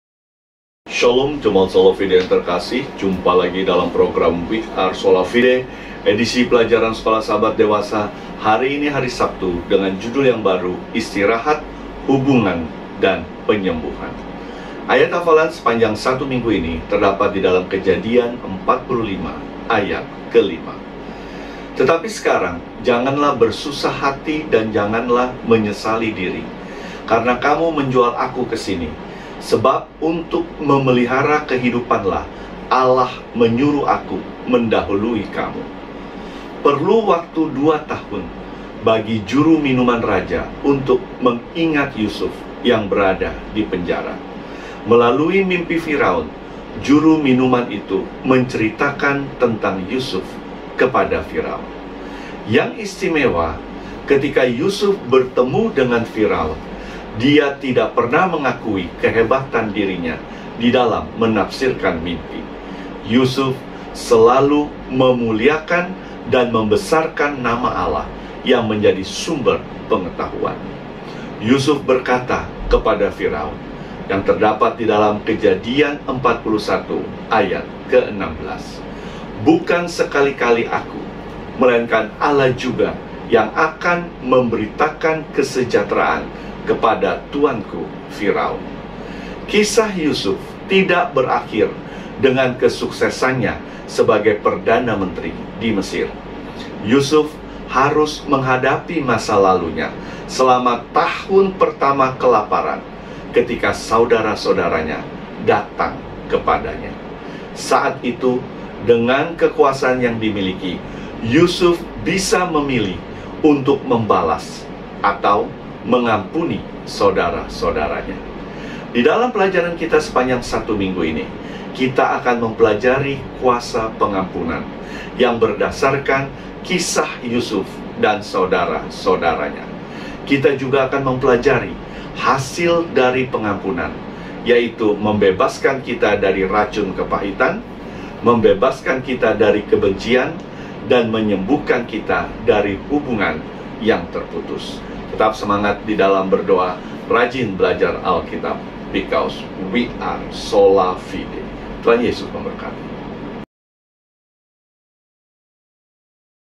Pelajaran SS Dewasa 7 August 2021, "Istirahat, Hubungan, dan Penyembuhan"
Pelajaran SS Dewasa 7 August 2021,
"Istirahat, Hubungan, dan Penyembuhan"
Pdt. Heince Rusli, GMAHK SOLAFIDE - BSD